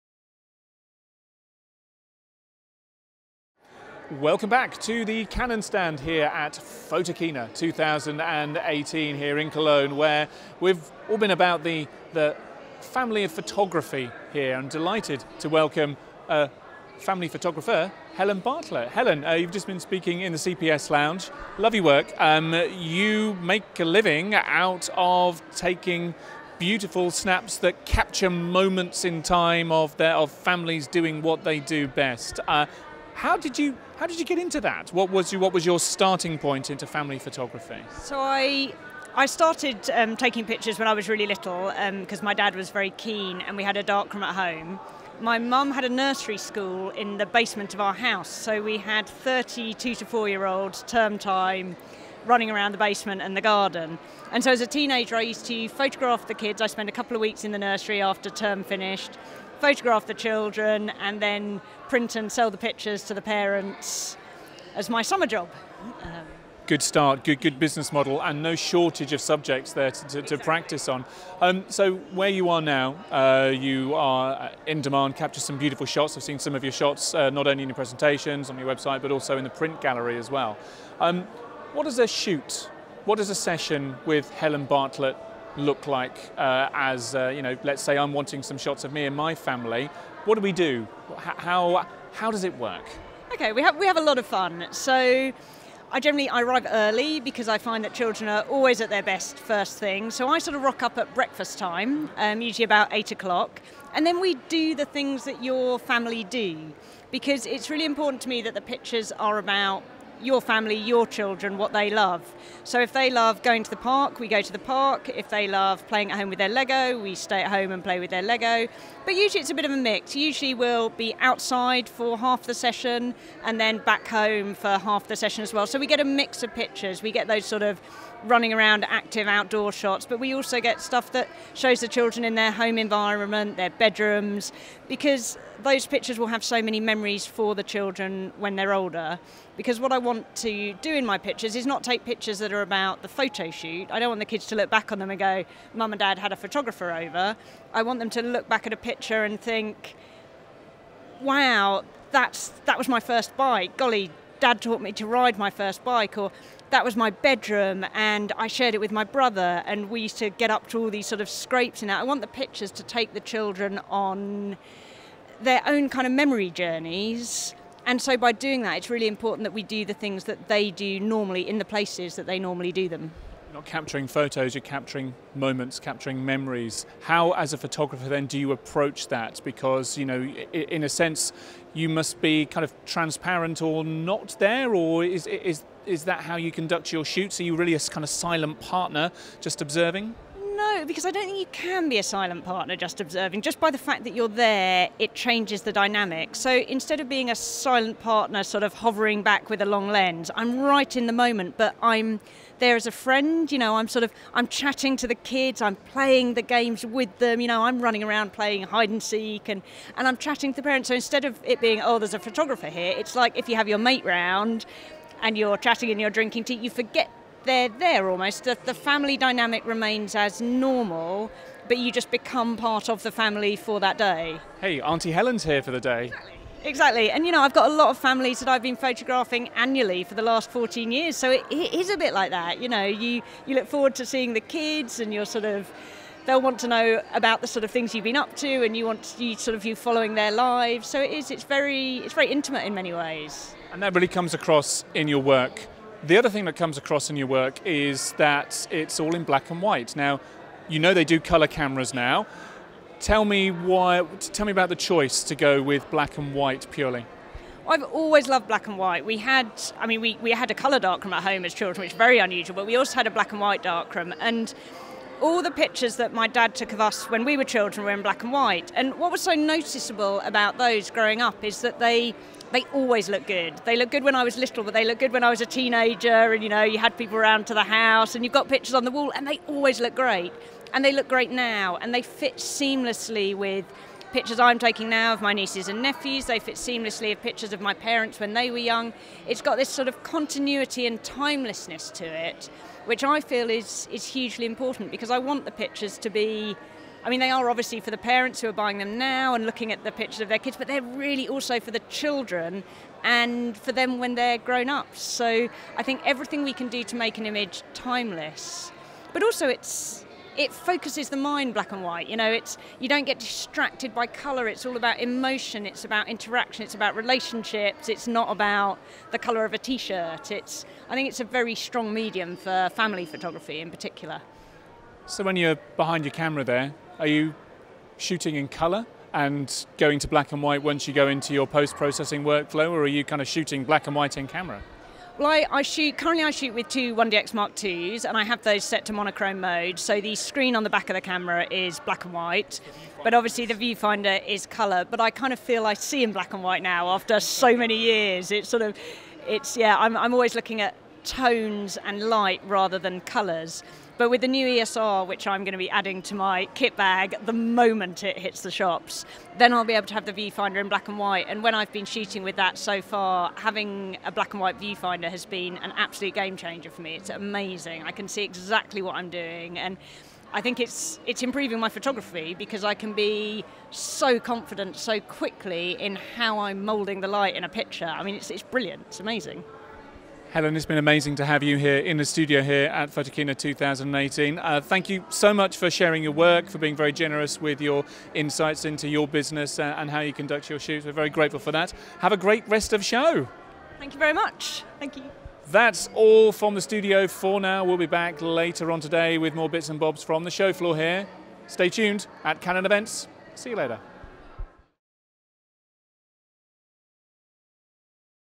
Photokina 2018 Conversations: Canon Ambassador Helen Bartlett
We talk to Canon Ambassador Helen Bartlett at Photokina 2018.